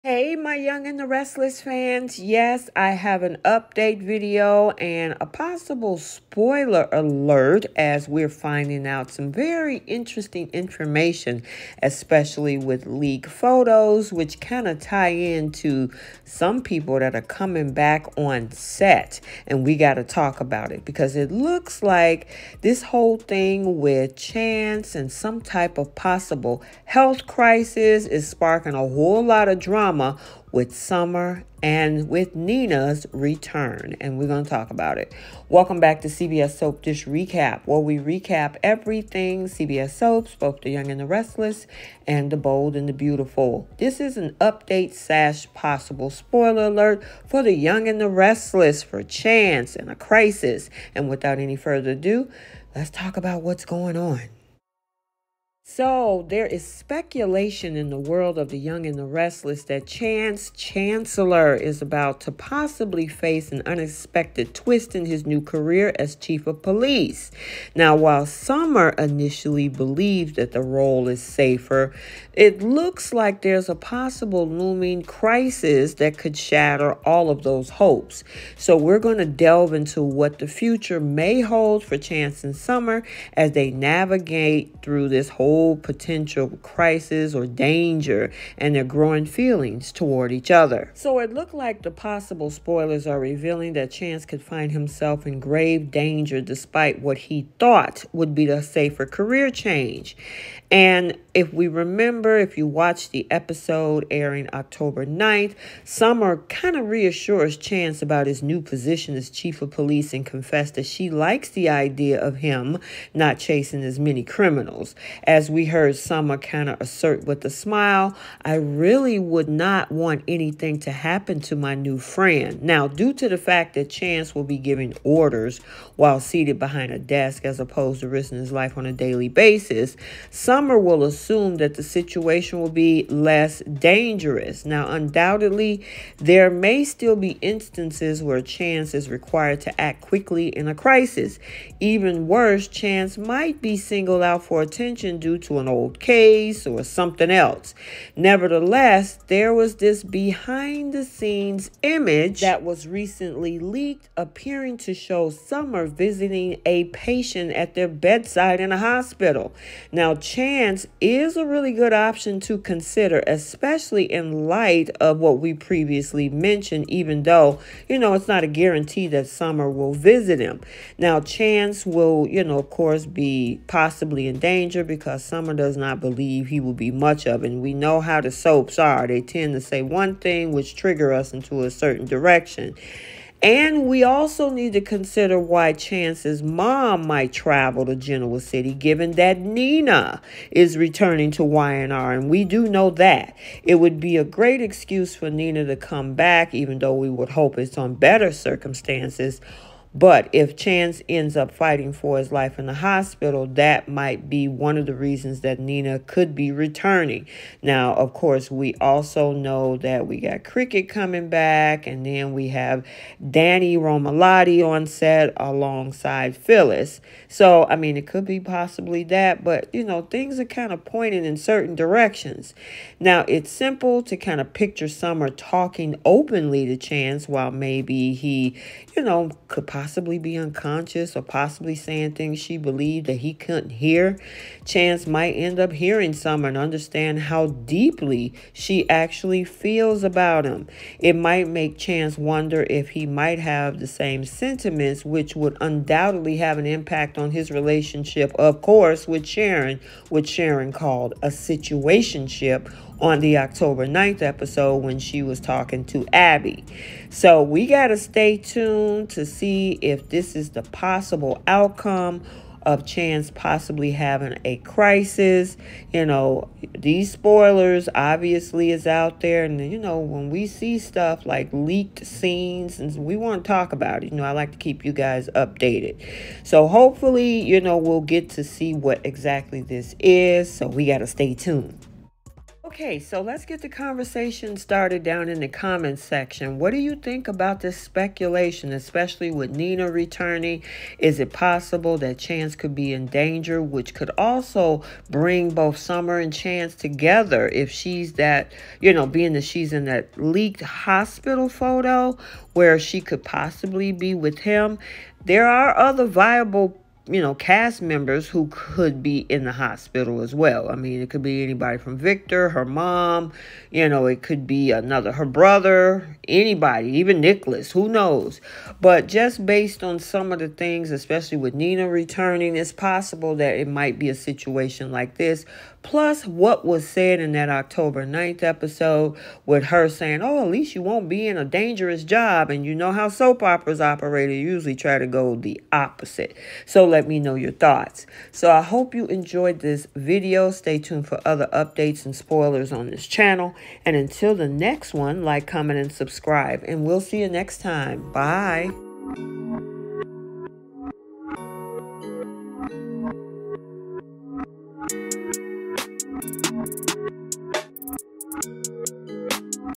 Y&R Exclusive: Chance's Possible Hospital Saga! Summer at Chance's Bedside with Nina’s Return?
"🚨 Y&R Exclusive: Chance's Hospital Drama Unveiled! 😷 Summer's Emotional Vigil by His Side & Nina's Shocking Return? Get the Inside Scoop on The Young and the Restless! 🔥 Don't miss this gripping update on Chance's fate and the unexpected twists awaiting you! 📺 YR SoapOpera ChanceHospitalSaga NinaReturns"

Announcing Fan Merchandise!! The Young & The Restless and The Bold & The Beautiful!!